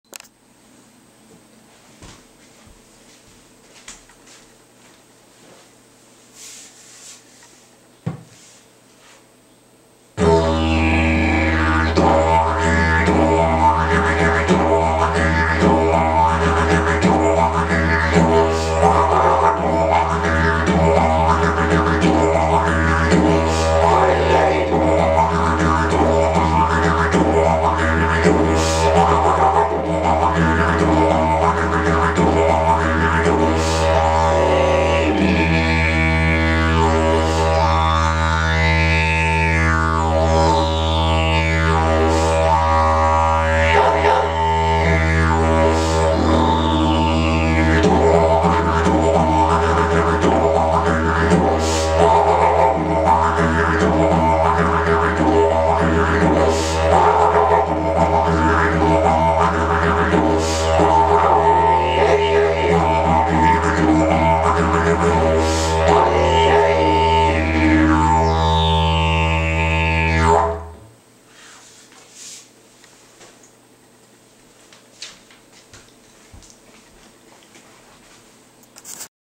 Didgeridoo E made of bird-cherry tree
Didgeridoo E made of bird-cherry tree played by Janne Murevärn at
Dreamtime Didgeridoo.